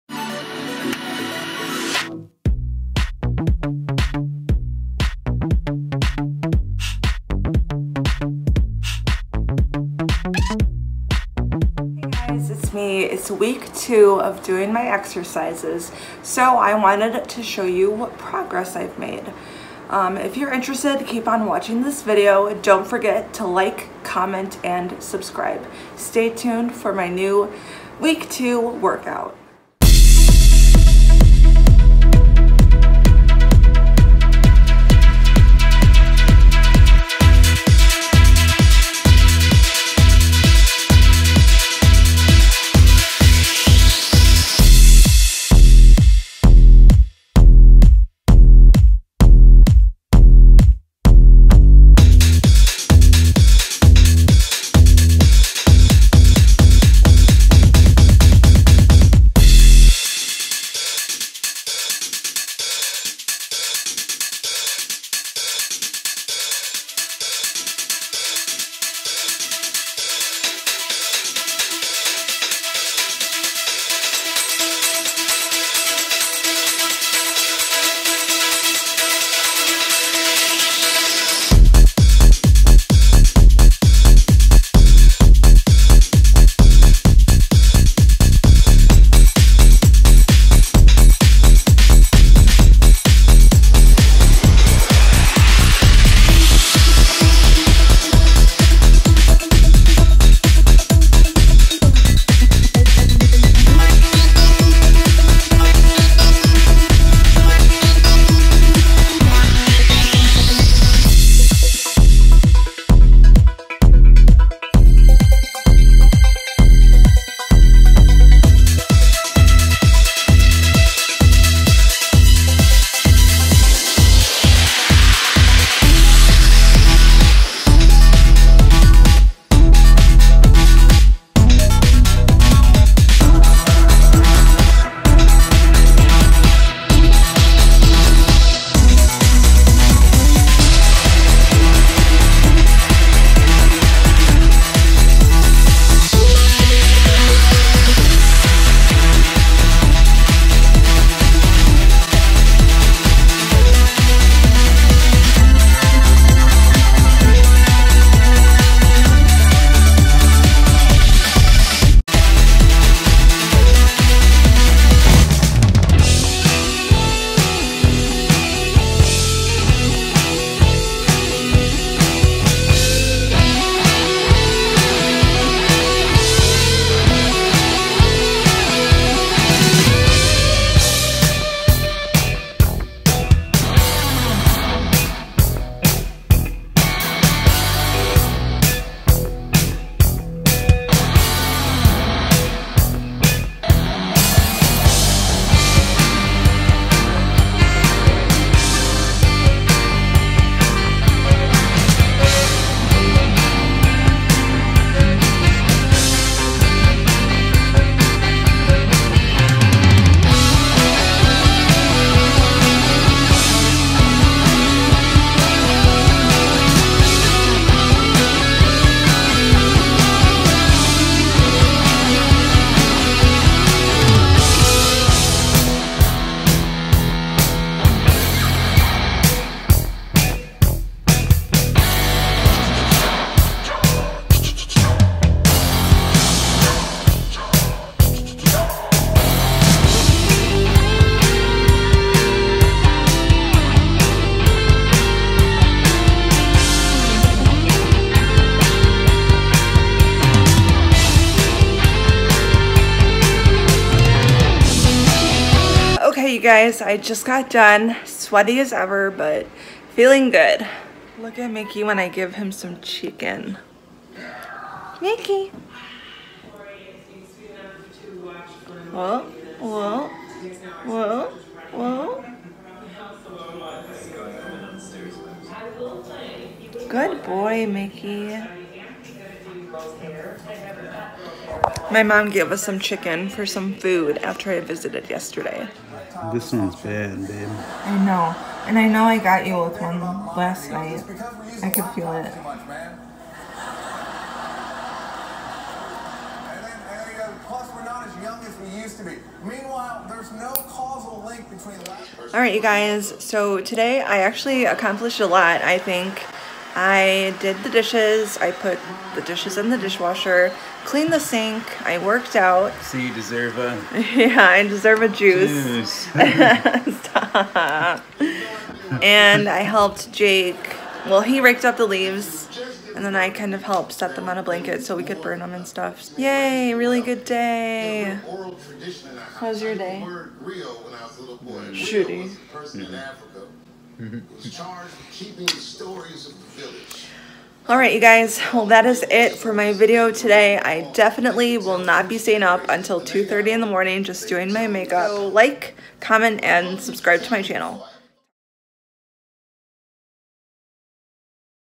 BATSHTKZ -- AMATEUR WORKOUT ((week two))
Thanks for visiting my channel, you guys!  This is my (hopefully) improved workout for Week 2! I've been keeping up very steadily and slowly straining muscles I haven't used in months! It feels good! XOXOXO, Kayla